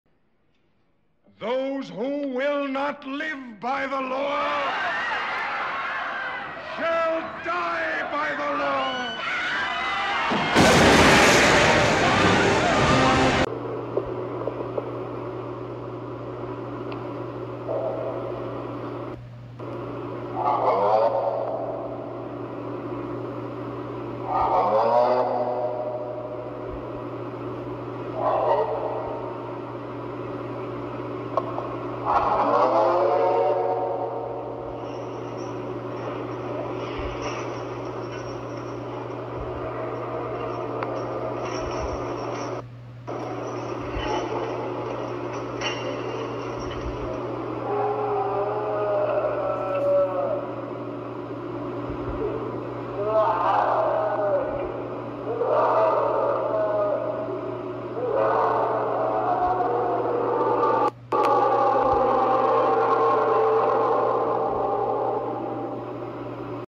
"The Euphrates River Mystery Unveiling the Voice of a Fallen Angel" God's Warning
In this video, we explore the mystery behind a shocking recording of a voice claimed to belong to a fallen angel under the Euphrates River. Is it possible that a supernatural entity is lurking in the depths of the river?

Our team has conducted extensive research and analysis to unveil the truth behind this chilling phenomenon. From examining ancient texts to analyzing the audio recording, we leave no stone unturned in our quest for answers.

Join us on this intriguing journey as we delve into the depths of the Euphrates River and seek to uncover the truth behind this supernatural voice.

If you're a fan of paranormal mysteries and unexplained phenomena, this video is a must-watch. Don't miss out on what could be the most shocking revelation of the year.

So, sit back, relax, and prepare to be amazed as we take you on an unforgettable journey into the unknown.